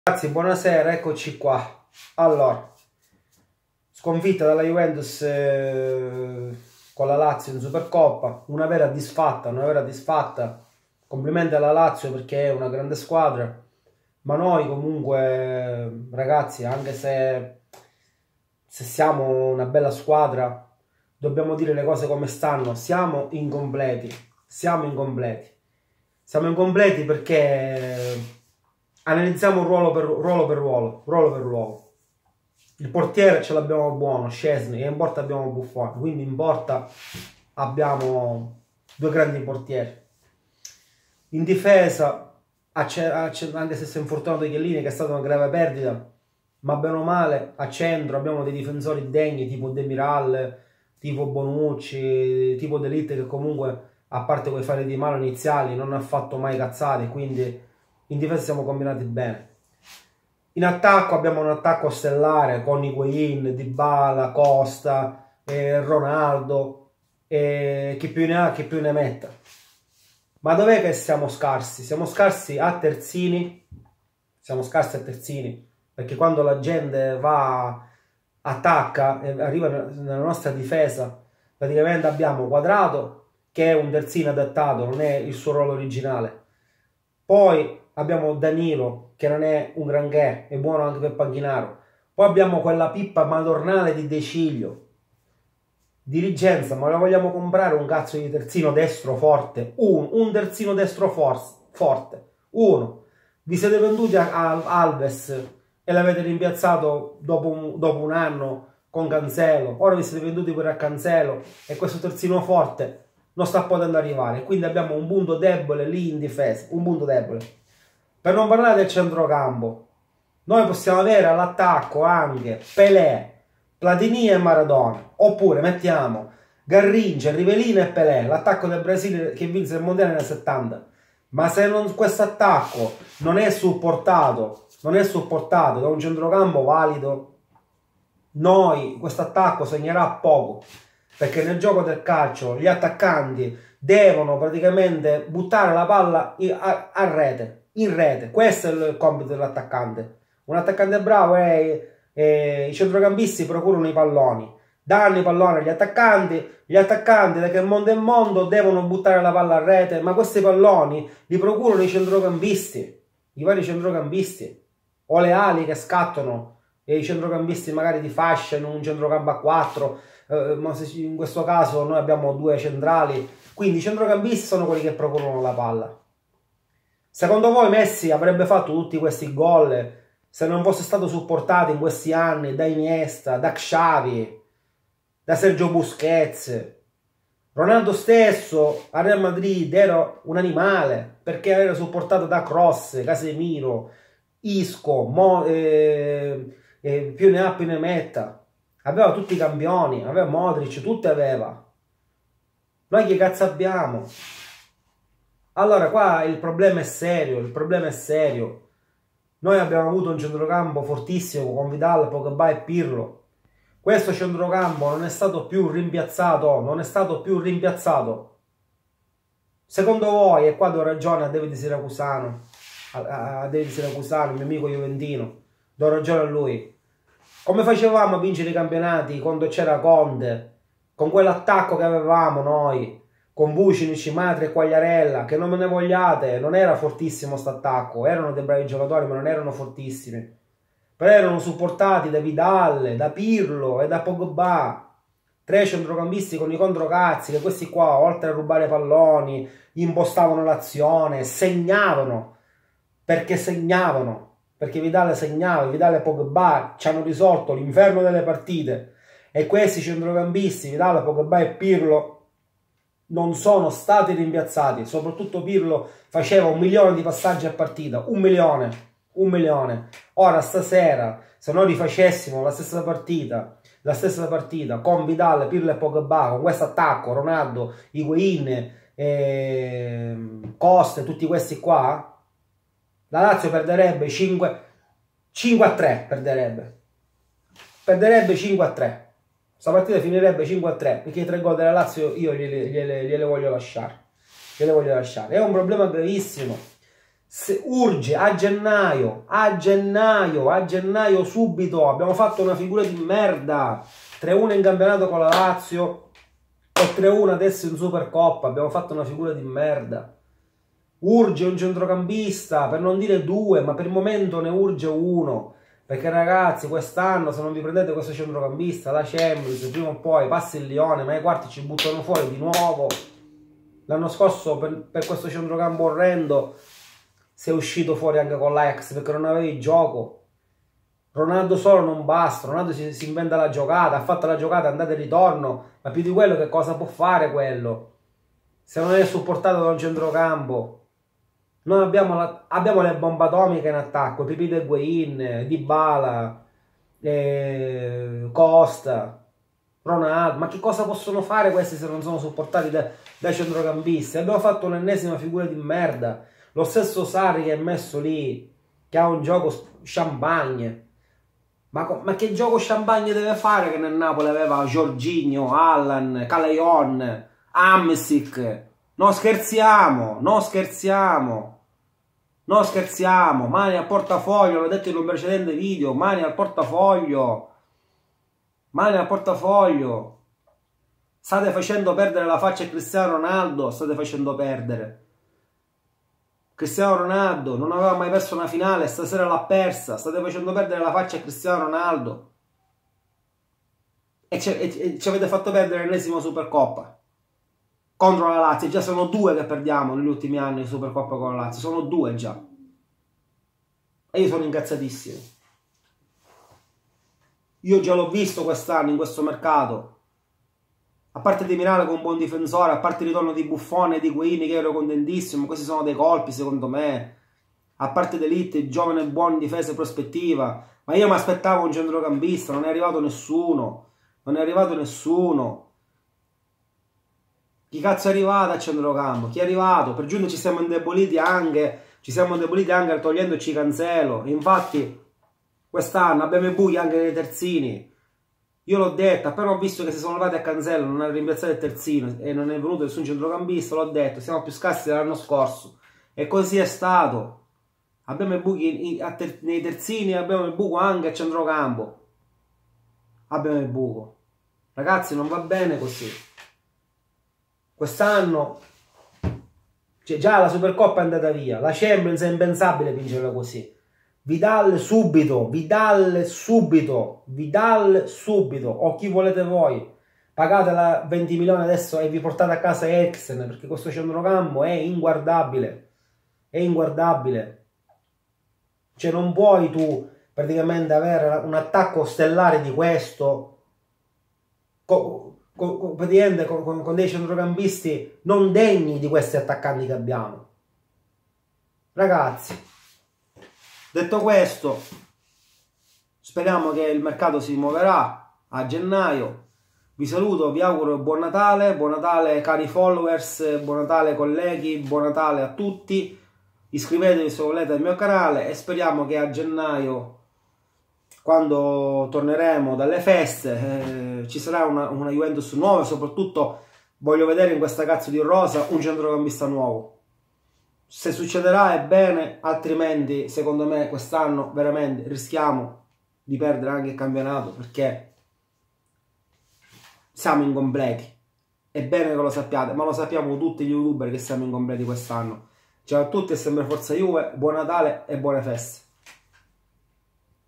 Ora basta..Vidal subito a gennaio..disfatta in supercoppa
con un attacco stellare ma senza centrocampo e senza terzini non si va da nessuna parte..